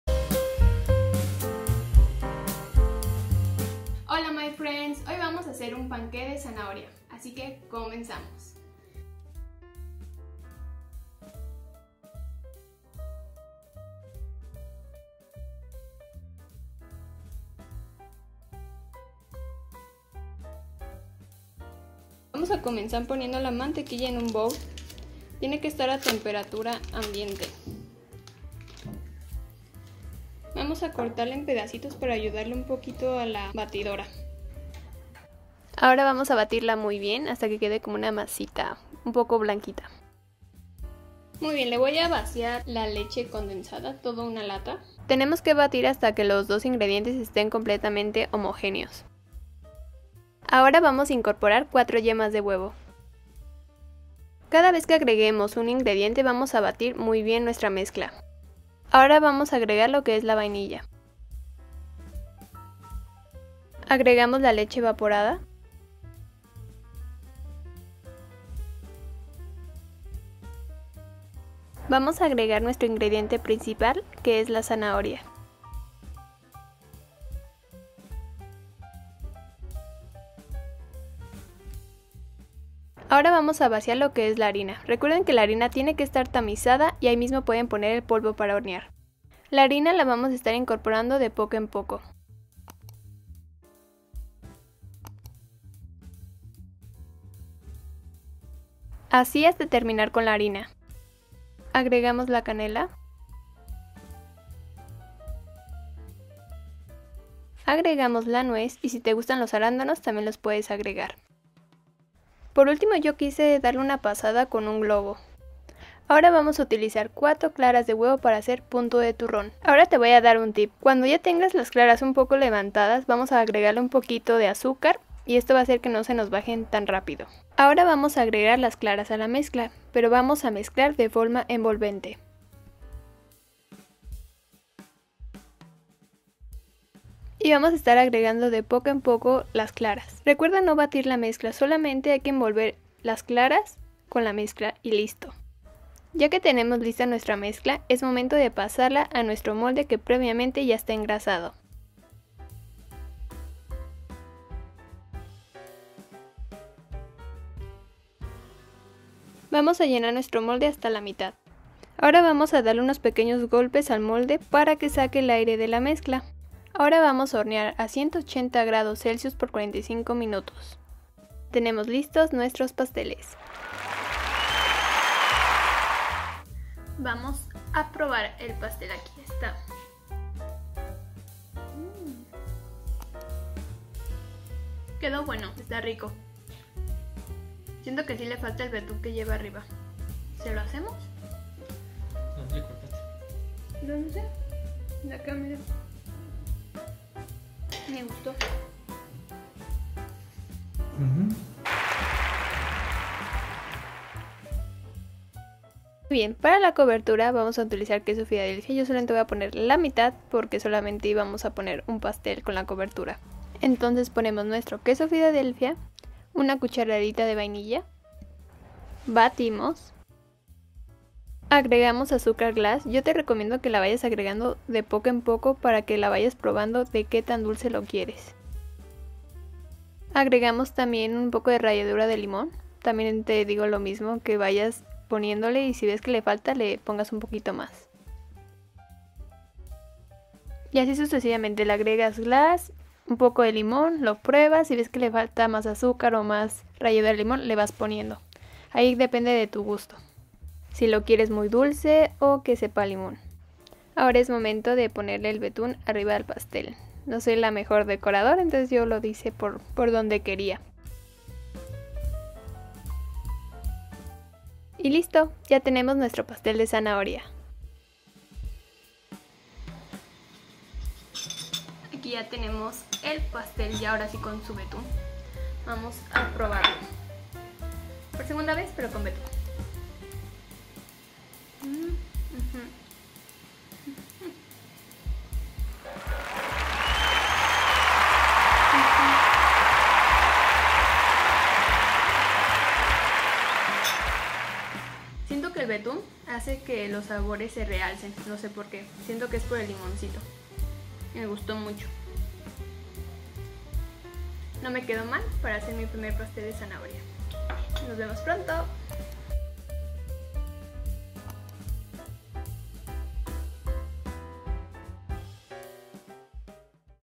PASTEL DE ZANAHORIA /PANQUÉ DE ZANAHORIA FÁCIL Y RAPIDO
Hola my friends hoy les quiero compartir una receta de como hacer un  pastel de zanahoria /panqué  de zanahoria, super fácil de hacer y muy rico, espero les guste el video no olviden suscribirse y activar la campanilla de notificación , nos vemos pronto con un nuevo video, chao 👋

cocina fácil
receta fácil
 cake
postre
 tarta